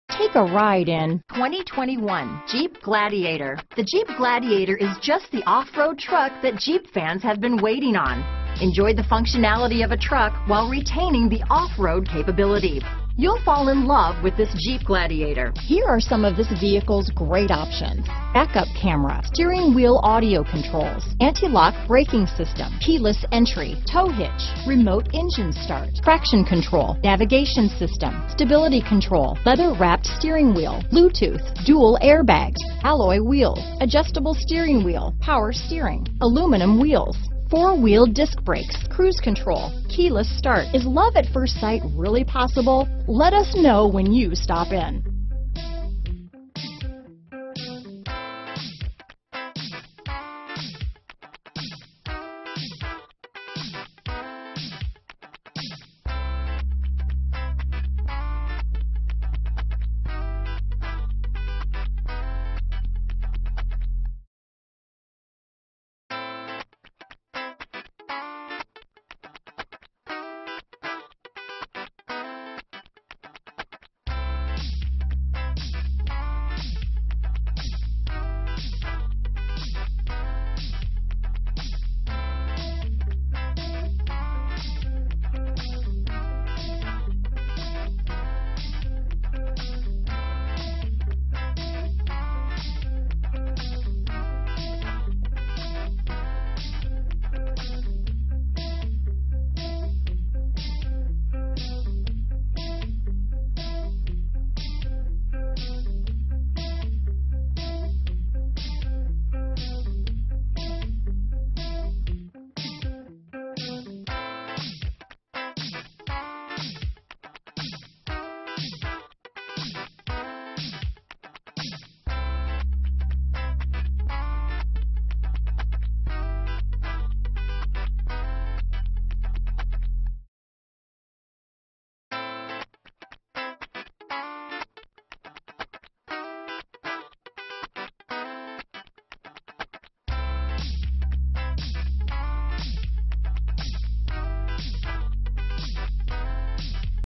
2021 Jeep Gladiator Orlando FL, Central Florida, Winter Park, Windermere, Clermont, FL M0065
Bright White Clearcoat New 2021 Jeep Gladiator available in Orlando, Florida at Orlando Dodge. Servicing the Central Florida, Winter Park, Windermere, Clermont, FL area.
2021 Jeep Gladiator High Altitude - Stock#: M0065 - VIN#: 1C6HJTFG9ML505956

Orlando Dodge
4101 W. Colonial Dr